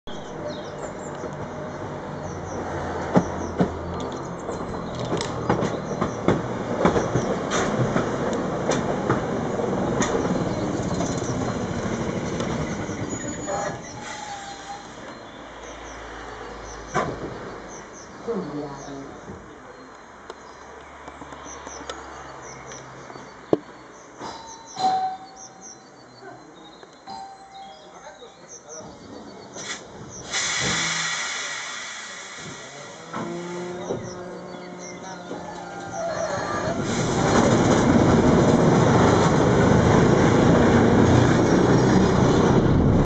BKV 60-as villamos - Gyöngyvirág úti kereszt 2013.04.17 19:08:10
Az 53-63 és az 55-65 pályaszámú osztrák SGP járművek keresztezik egymást a Gyöngyvirág út állomáson a budapesti 60-as (Fogaskerekű) villamos vonalán.
The number 53-63 & 54-64 Austrian Simmering-Graz Pauker cog-wheel trams cross each other at Gyöngyvirág street station on Budapest tram line 60.